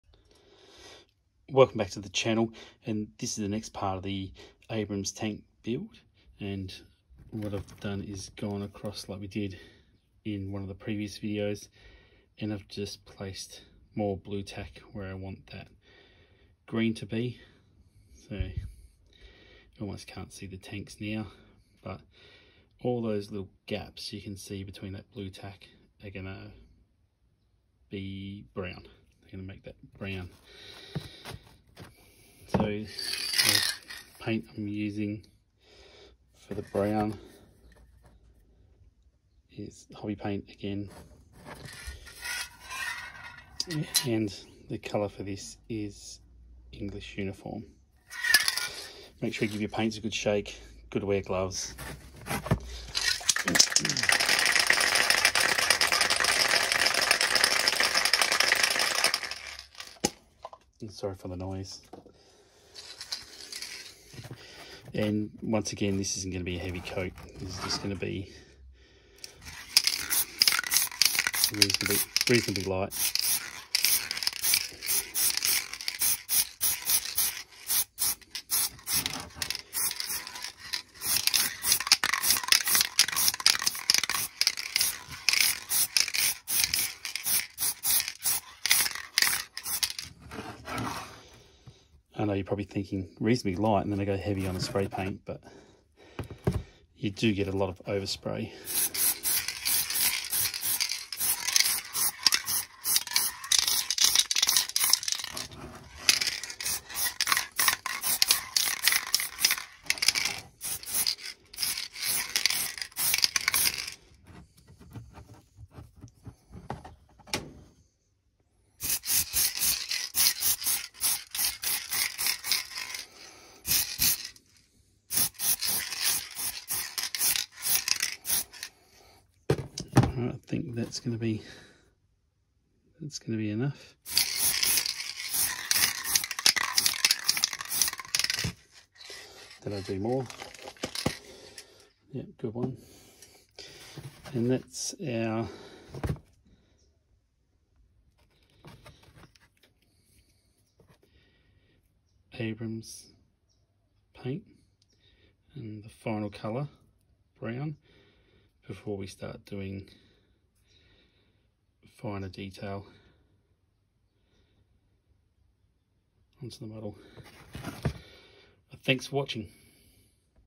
GHQ Micro Armour build spray painting the brown on the tanks, part 6. 1/285 or 6mm.
Spraying the brown on the GHQ M1A2 1/285 models.